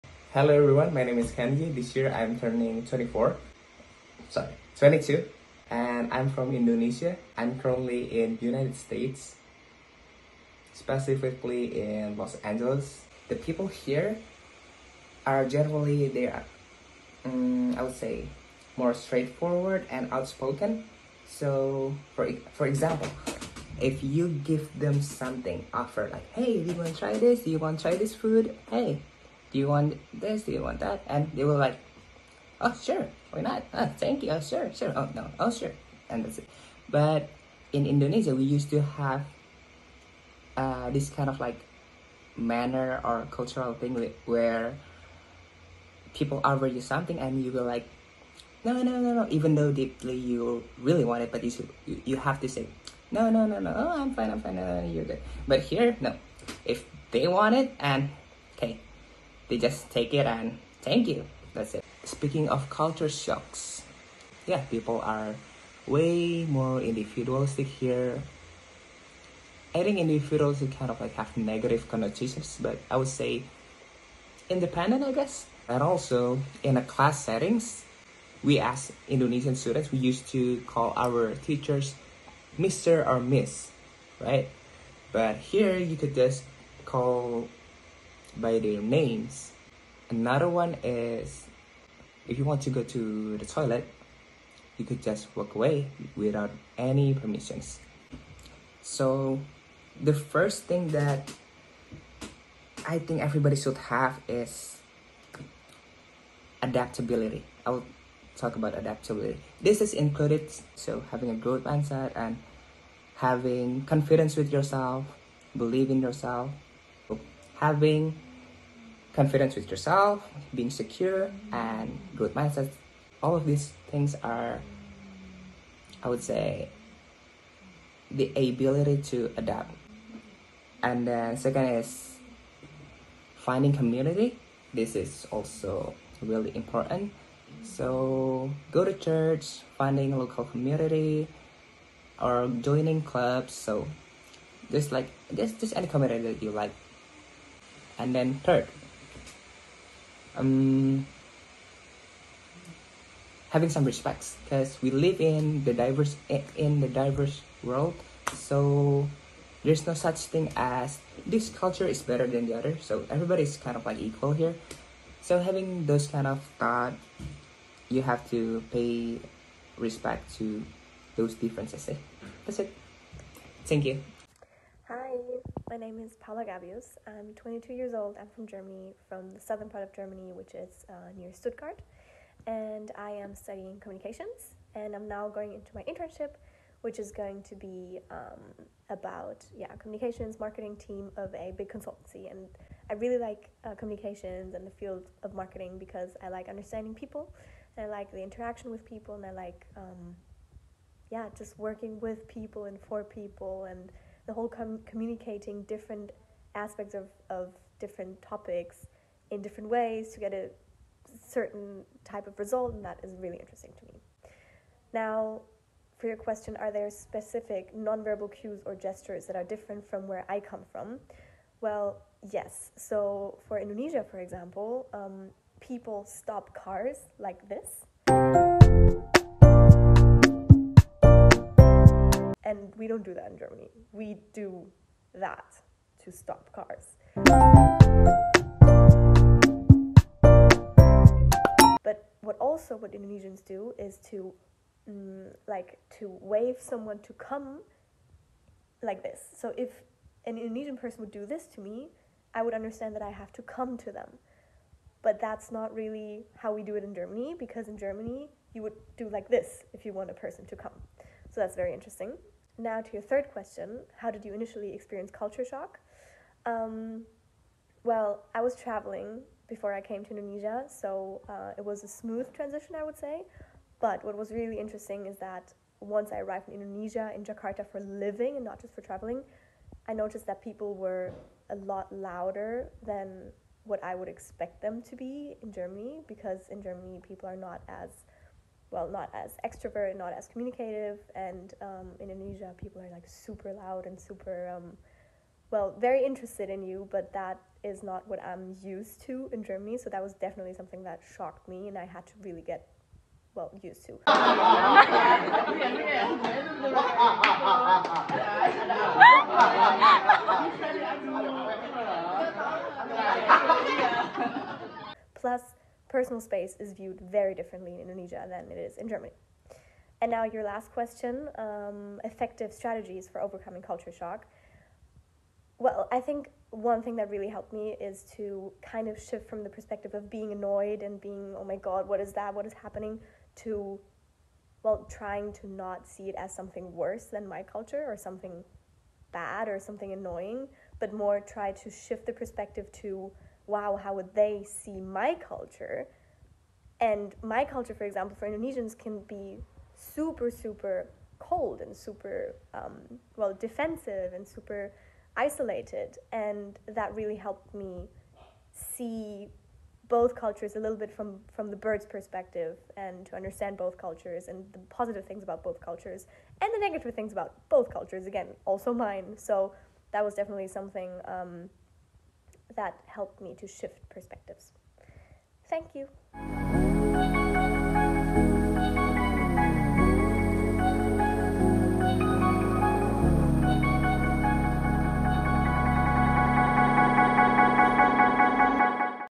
Final Exam Intercultural Communication - Culture Shock
This video is made to fulfill the requirements for the final semester exam of Intercultural Communication. We have chosen the topic of culture shock for our video. We would like to express our gratitude to our lecturer for the guidance provided to us from the beginning of the learning process in the first semester until the end of the semester. Also, a big thank you to the speakers who have assisted us in creating this video.

Lecturer :
Mrs. Susan Davis

Members :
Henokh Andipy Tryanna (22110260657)
Abiashafa Nada Ramadhani Wikrama (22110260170)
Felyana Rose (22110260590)
Muhammad Alief Agung Alim Valerie (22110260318)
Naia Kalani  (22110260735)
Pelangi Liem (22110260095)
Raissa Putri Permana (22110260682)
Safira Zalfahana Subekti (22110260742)

Interviewees :
Kenzie Zhang
Paula Sophie Gabius